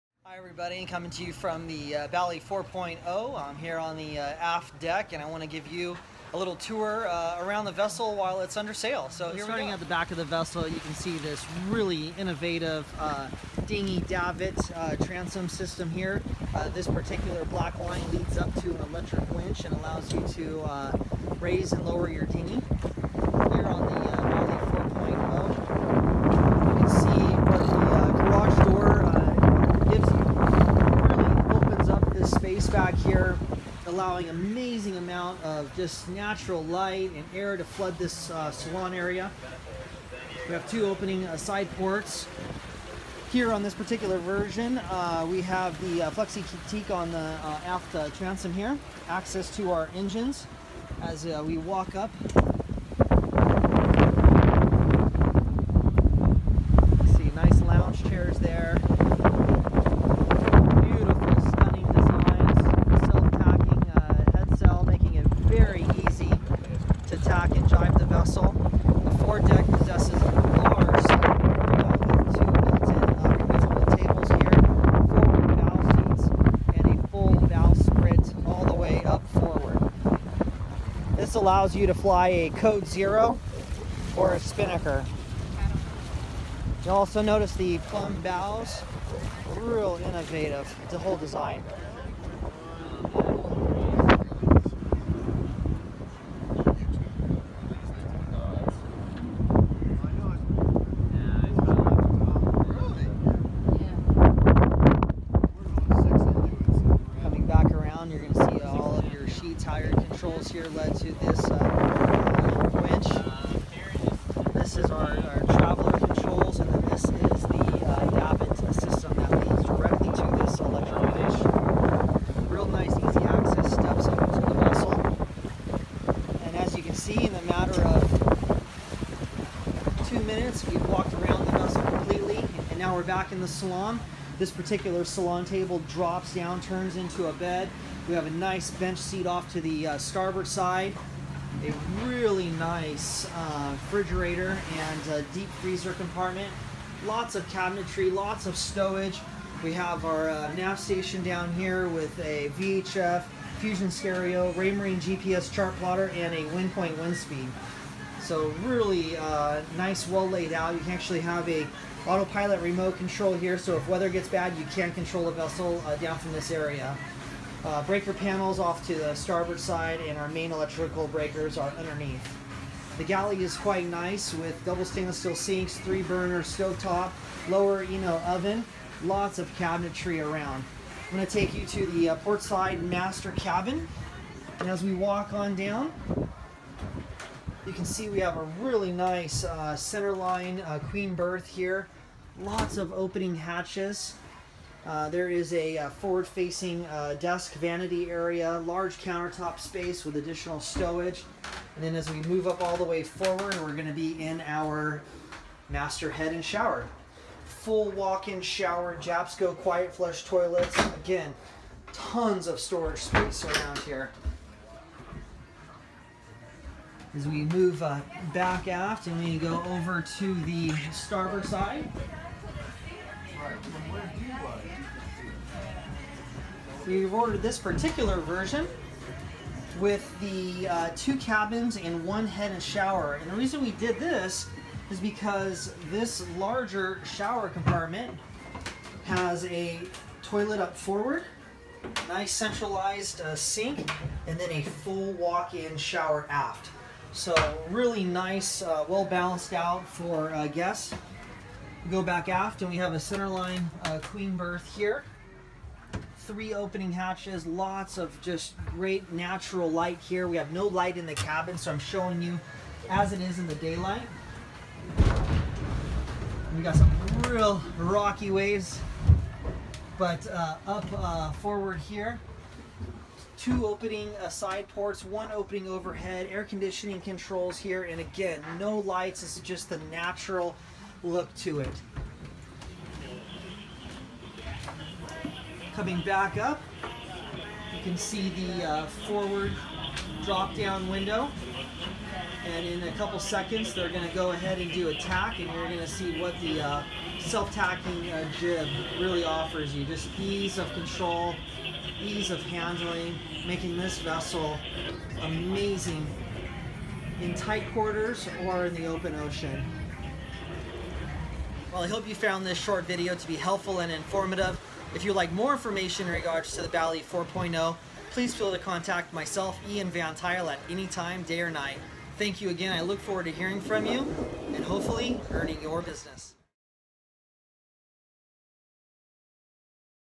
Video Walkthrough on the Currently For Sale New 2017 Bali 4.0 under sail By: Ian Van Tuyl
This video is about Video Walkthrough on the Bali 4.0 under sail By: Ian Van Tuyl.
or
I look forward to hearing from you and hopefully earning your business.